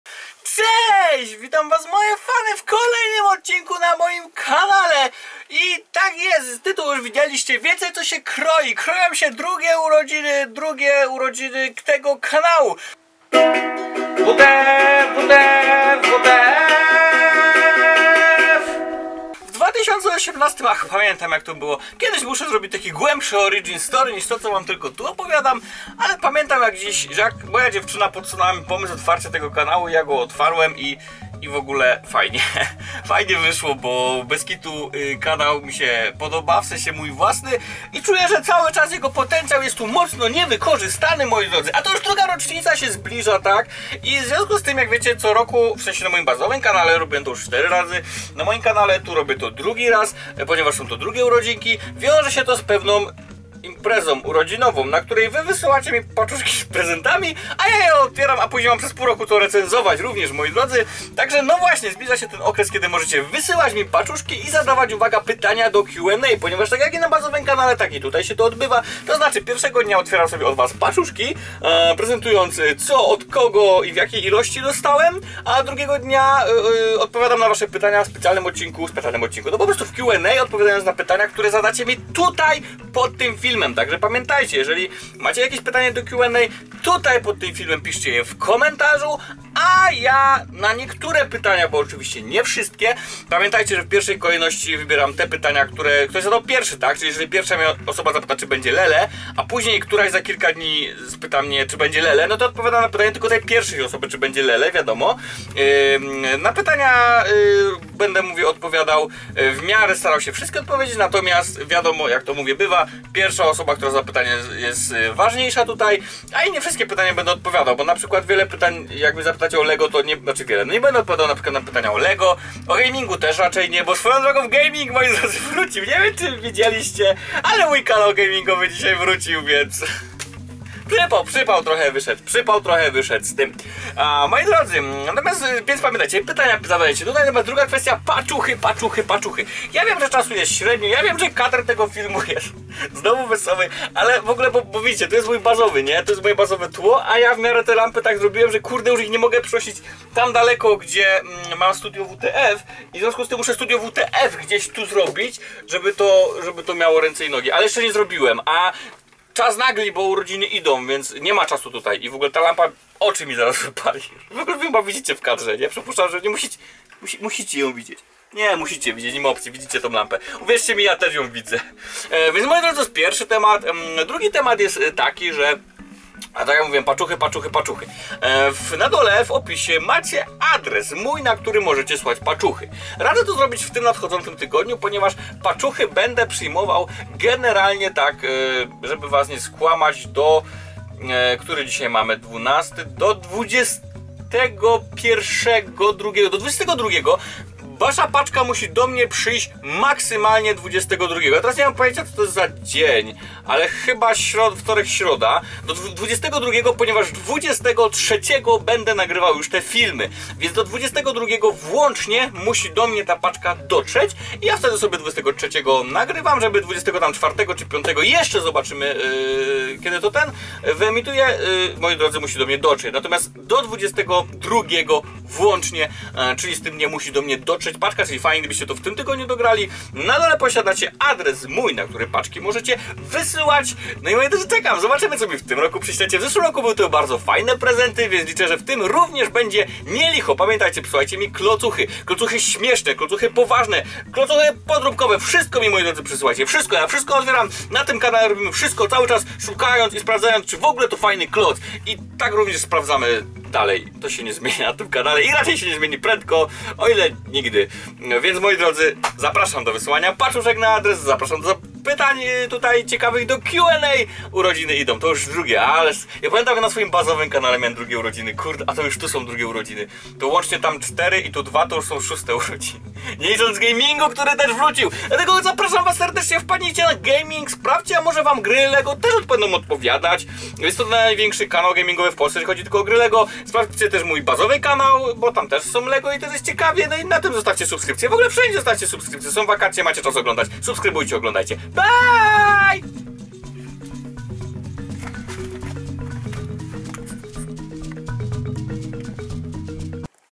Idą DRUGIE urodziny kanału ! + pytanie do Q&A
Siemka - drugie urodziny to już nie przelewki !
Paczuchy ślijcie więc na :
Szymon Mentlewicz
ul. Kr.K.Wielkiego 11
32-300 Olkusz

Nie zapomnijcie też dawać pytań w komentarzach pod tym filmem :)

Na filmik urodzinowy łapią się wszystkie paczki które dotrą do mnie do 22 lipca włącznie.
Pytania możecie zadawać do 23 lipca włącznie.